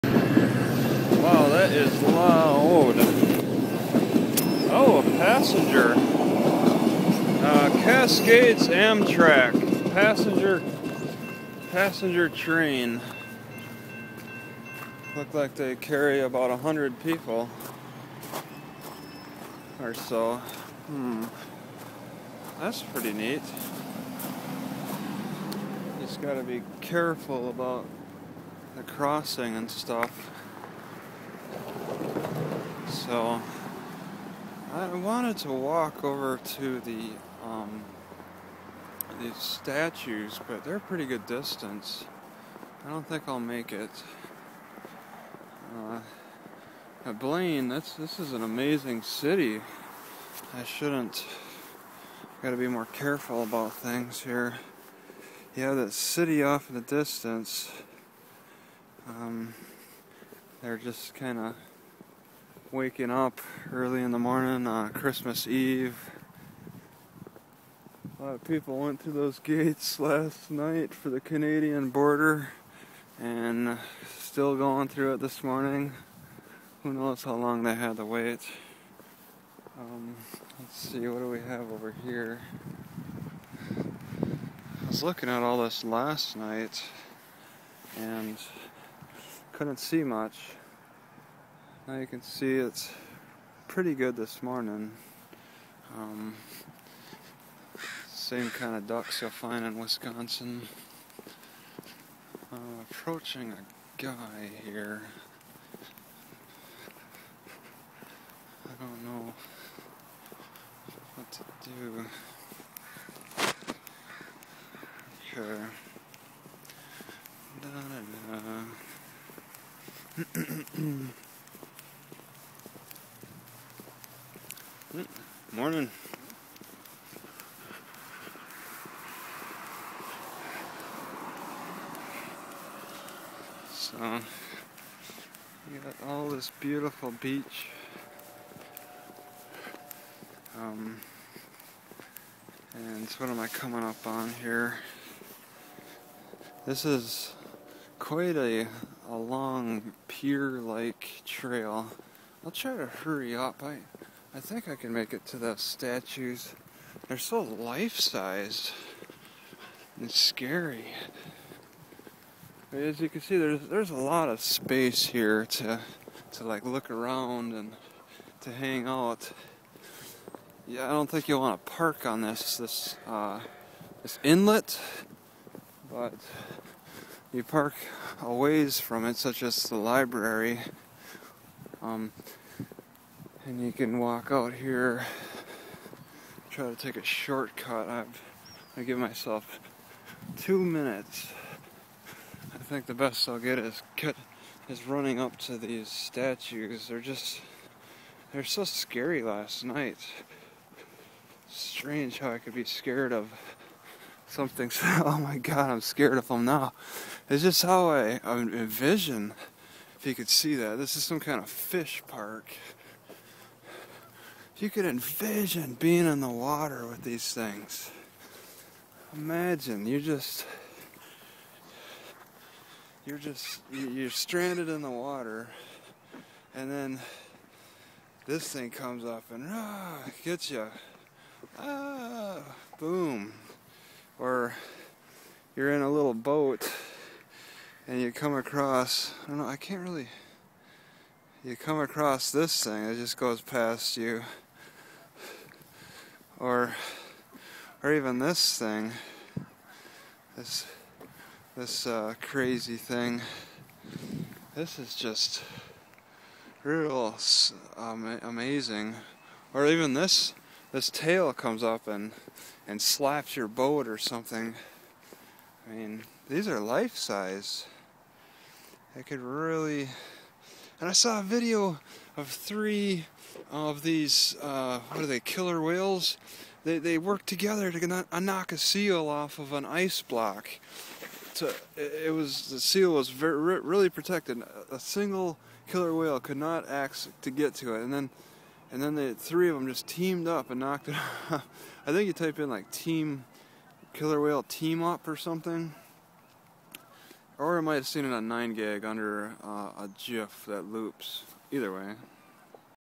Selfie with Killer Whales of Blaine, Washington
I walk over to this parj and discover the lifelike killer whales and get a little intimidated by them. These creatures are serious about hunting and will team up with each other to kill their prey.

I can only see myself being that seal and only thinking about my eventual doom when those whales coordinate a successful attack. Could these whales perform a capsizing to a stranded boat if they wanted to? I must stay away from these animals if I want to live.

The killer whales are statues in a park. Visitors can have their pictures taken with them. The killer whale is a social creature. Their social structure is matrilinea. Some species of killer whale are considered endangered. Orca is the species the killer whale falls under. In history, the orca were named by the Romans. Killer whales teeth are covered in enamel. A killer whale has a dorsal fin and saddle patch. Killer whale eyes can see above and below the water. Females mature at age 15. The female can go through menopause and live decades afterward. Females breed until age 40.

The average lifespan is about 50 years old and some can live to 80 or 90 years.

Killer whales practice jumping out of the water which is known as porpoising. Killer whales hunt a variety of prey. Killer whales have no predators which means they are apex predators. Each group of whales seem to prefer one type of food choice. There are some killer whales that hunt sharks. Squid and turtles are also prey. Killer whales are acrobatic. Killer whales communicate through clicks, whistles, and pulsed calls.
Blaine seems like a nice town. They have a lot of interesting things to look at for a tourist. I was highly entertained just walking down the wharf. I walked late at night and no one else was around.